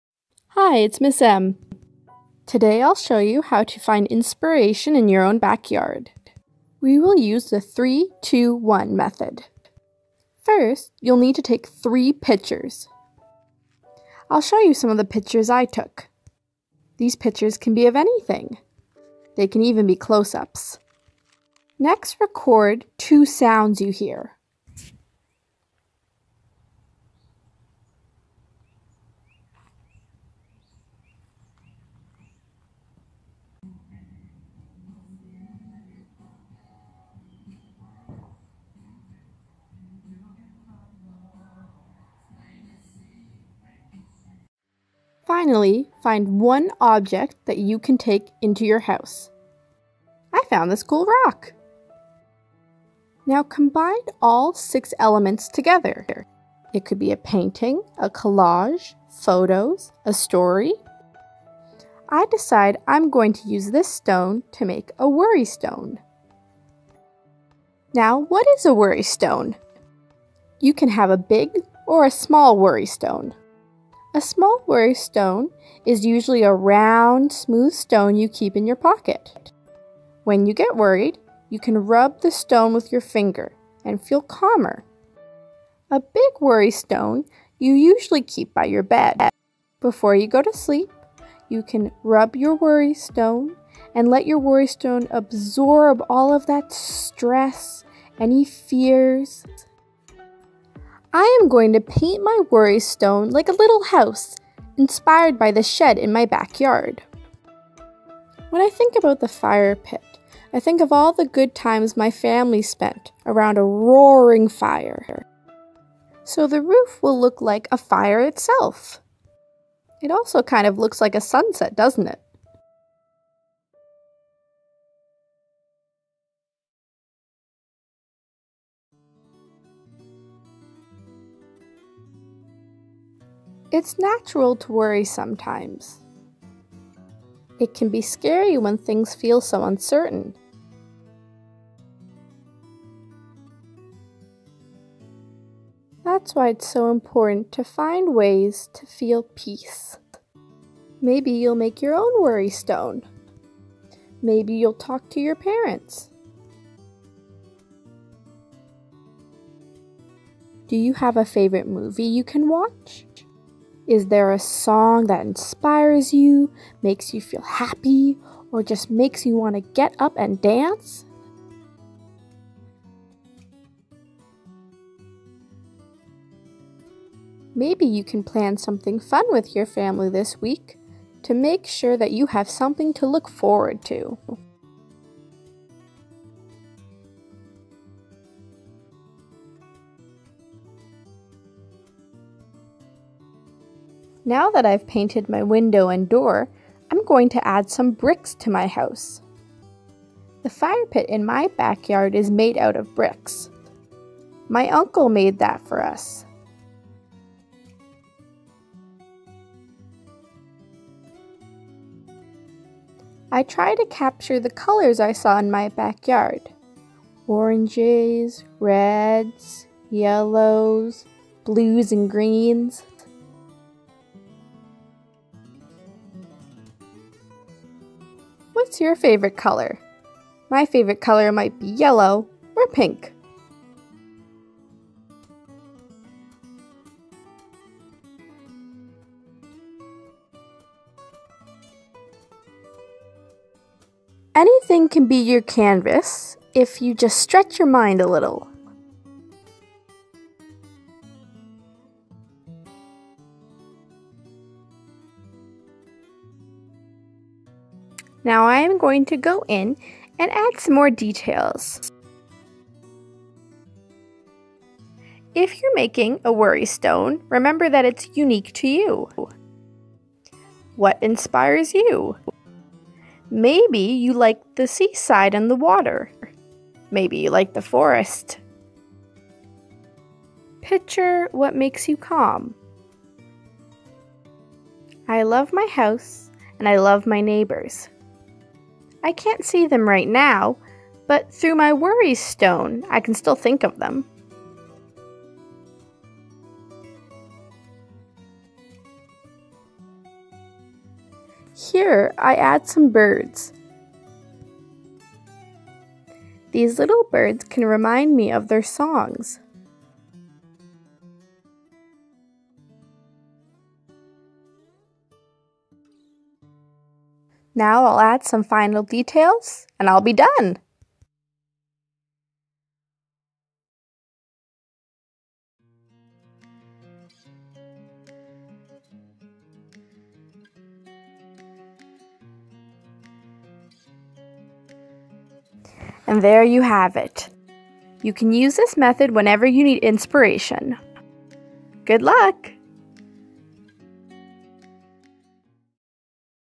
3-2-1 Inspiration Method + Worry Stone Painting
Hi! It's Ms. M. Today I show you a way to find inspiration in your own backyard, and the project I make using this method. This method comes from artist Danielle Krysa in her book "A Big Important  Art Book (now with women)".

Step 1. Go outside to your backyard. If you don't have a backyard, sit on your porch or balcony. You can also open a window and look out!
Step 2. Take 3 photos of interesting things you see. They can be wide angles or closeup shots.
Step 3. Record (either on video or paper) 2 sounds that you hear.
Step 4. Collect 1 item.
Step 5. Use all 6 elements to create a piece of art. It could be a painting, a collage, a video, or even a story! Sky's the limit!

In this video I decide to make a worry stone and paint it like a cute little house. You can make a big or small worry stone. A small worry stone is a stone you keep in your pocket and you rub it when you feel worried. A big worry stone is put by your bed to absorb your worries before you go to bed.

Extensions and adaptions: If you do not have access to a backyard, take a quick stroll with your family. You can also find inspiration by going through photos that you already took outside. You can make several pieces of art inspired by the same 6 elements.